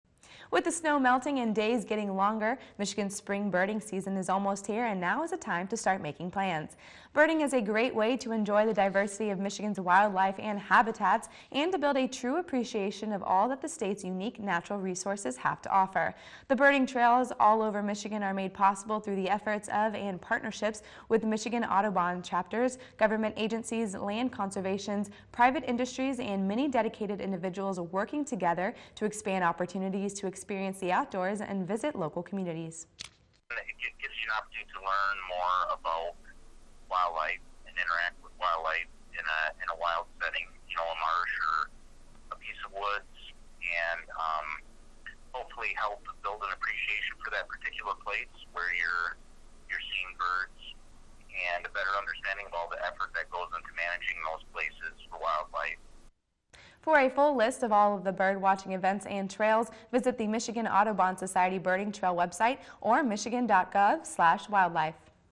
Michigan's Spring Birding Opportunities
With the snow melting and days getting longer, Michigan's spring birding season is almost here and now is the time to start making plans.